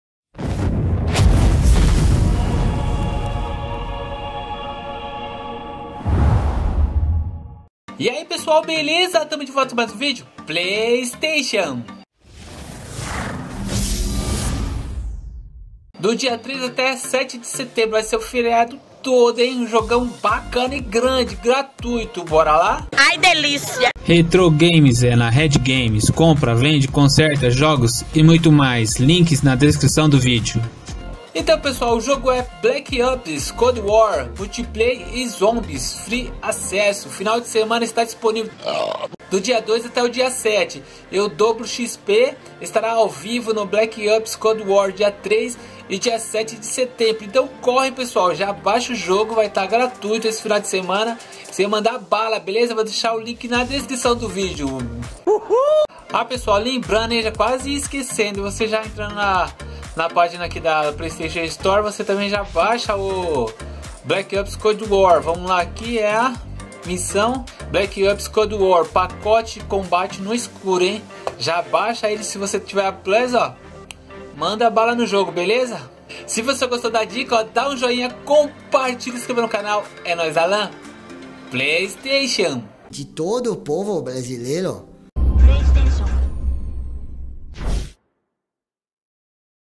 OBA JOGÃO GRÁTIS PARA ESSE FINAL DE SEMANA DE FERIADÃO PROLONGADO PLAYSTATION
►FONTES:  TWITTER
PLAYSTATION  Call of Duty®: Black Ops Cold War - Pacote de Combate (No Escuro)
Call of Duty®: Black Ops Cold War - Edição Padrão

Muito obrigado.

Eu estou usando o Ganhe na Tela para ganhar créditos para celular pré-pago! Use